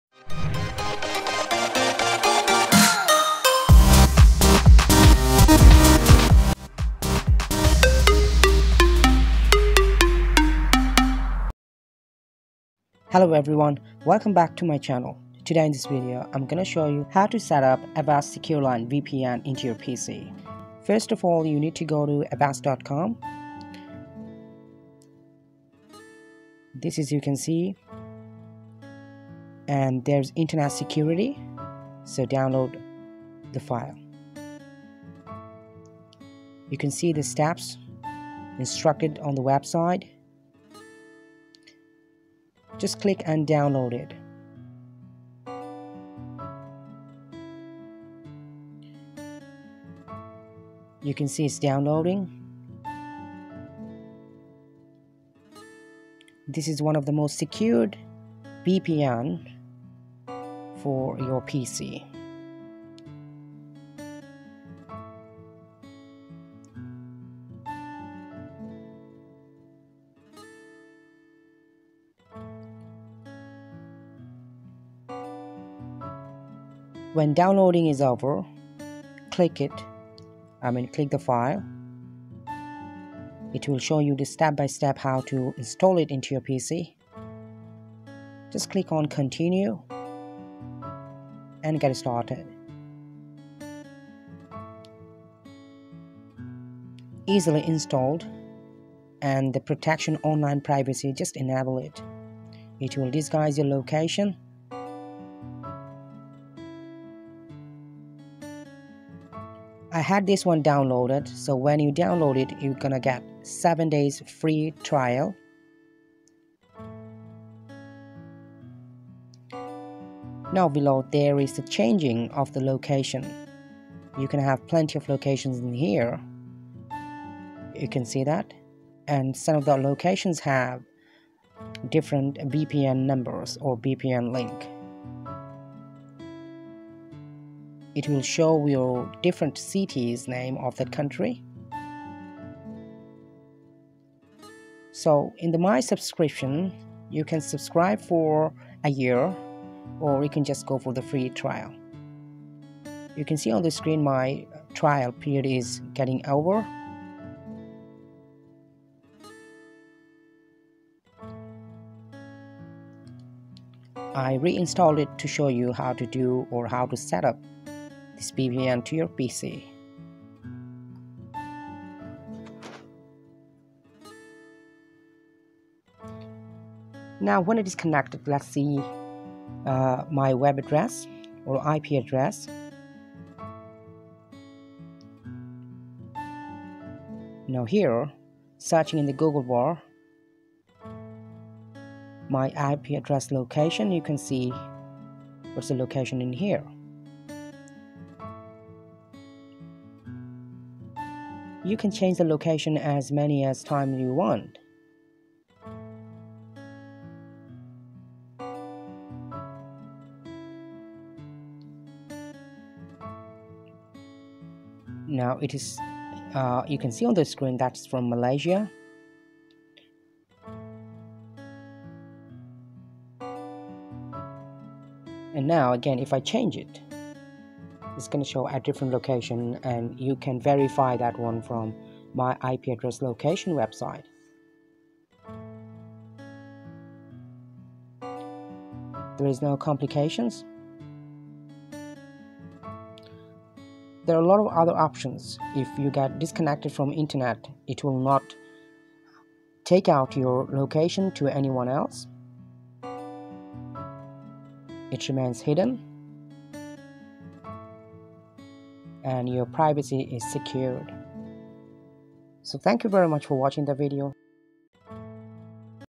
How to Install Avast SecureLine VPN
this video guides you how to install avast secureline vpn into your pc. It is one of the most popular vpn for pc, easy to install and hide location perfectly. So download, install, and enjoy your browsing. thank you.